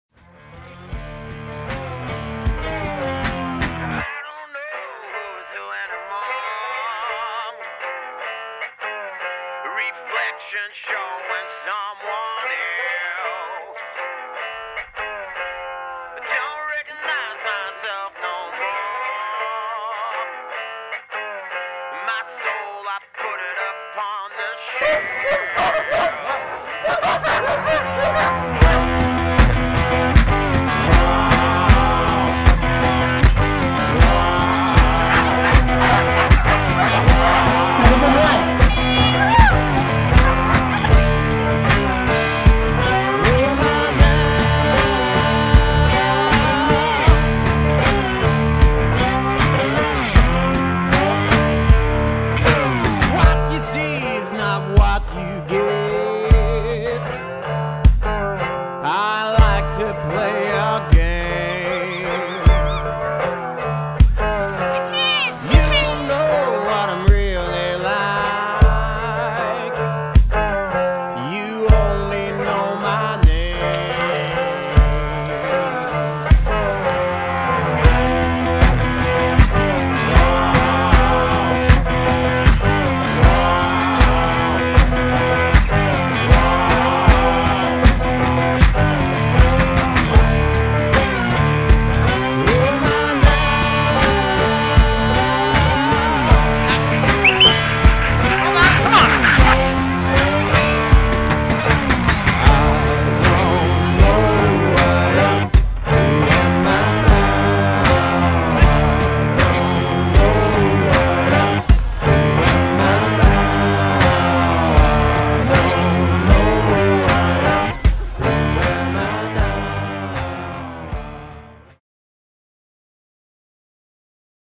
DinnerPlain SledDogChallenge Day1 2012 edited
The Dinner Plain Sled Dog Challenge 2012- Day 1
The 19th annual Sled Dog Challenge held in Dinner Plain in the Victorian high country is the oldest sled dog race on snow in Australia